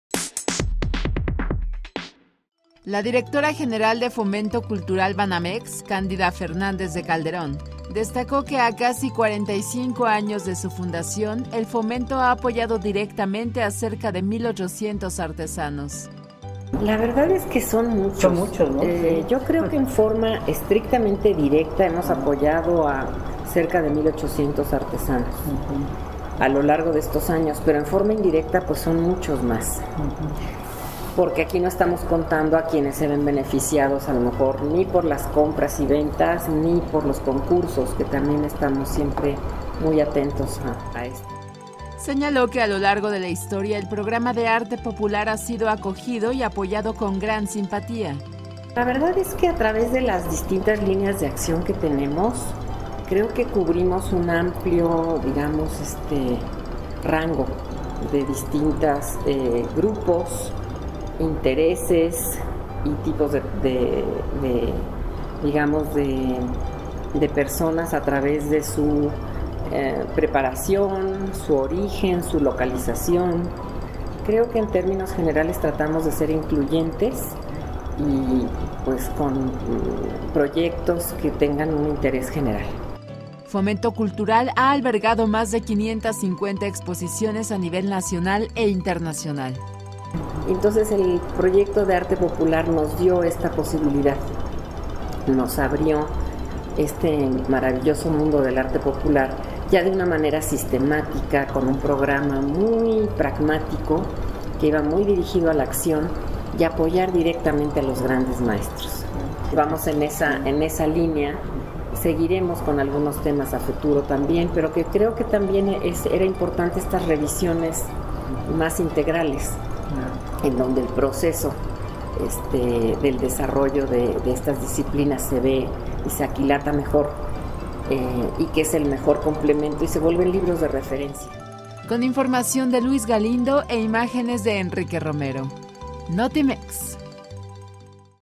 Fomento Cultural brinda apoyo a artesanos
El Fomento Cultural Banamex ha albergado más de 550 exposiciones a nivel nacional e internacional